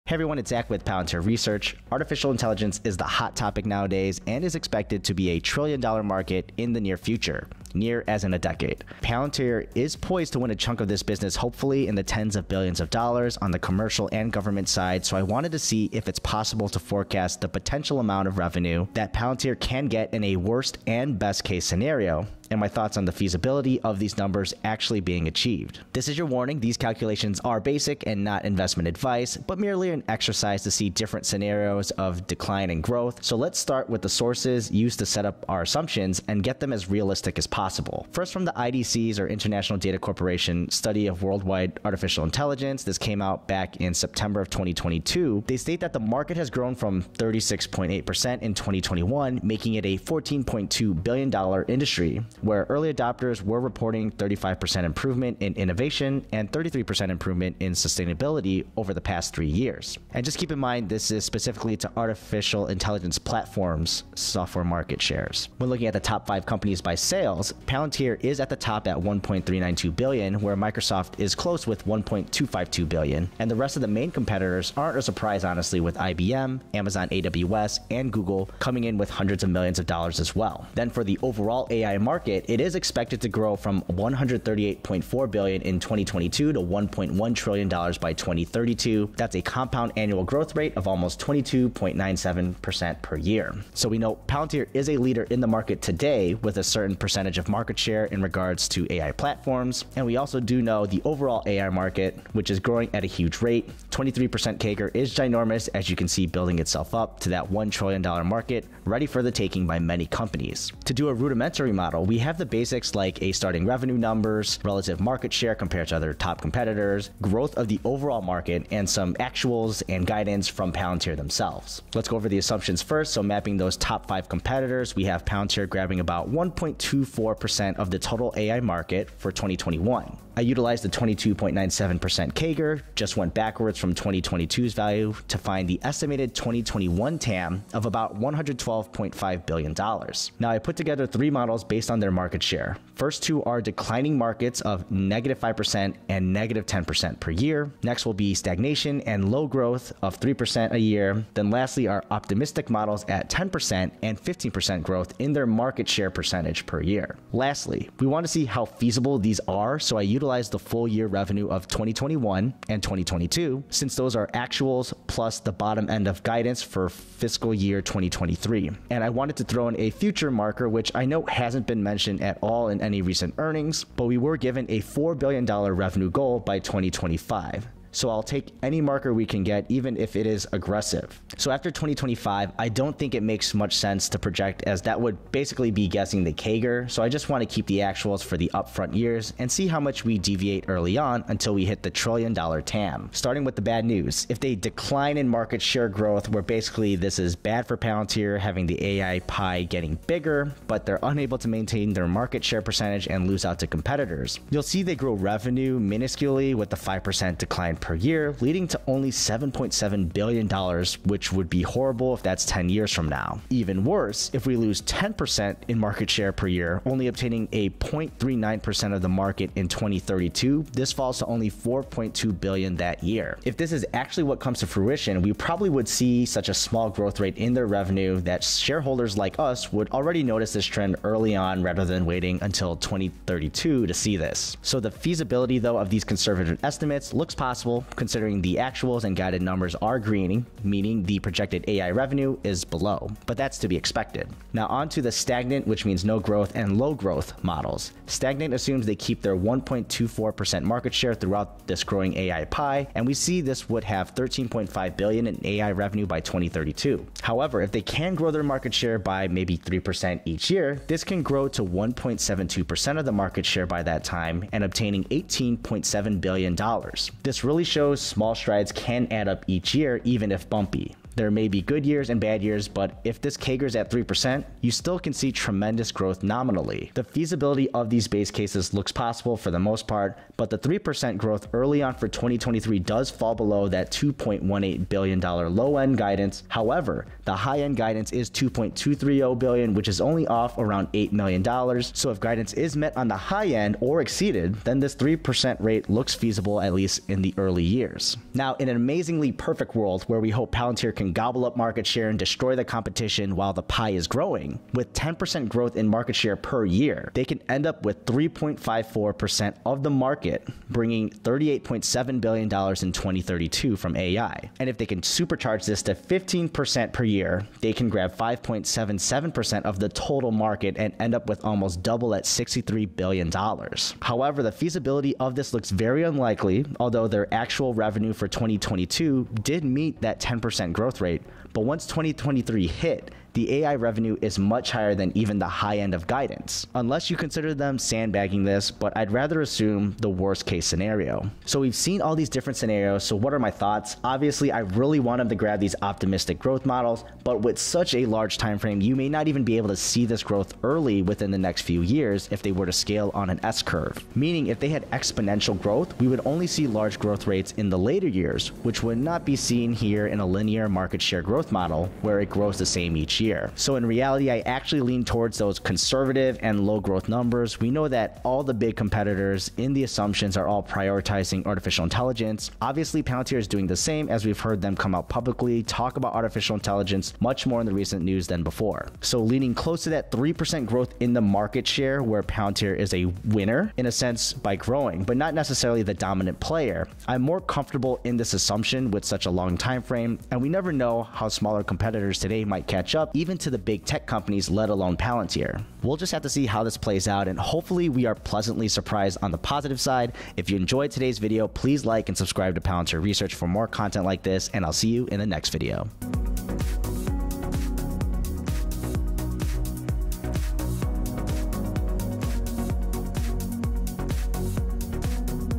Palantir Will WIN Billions in the Artificial Intelligence (AI) Race | PLTR Stock
Artificial Intelligence (AI) is the hot topic in tech today and all of big tech is competing to be the market leader of this future industry. Can Palantir win BILLIONS from everyone else?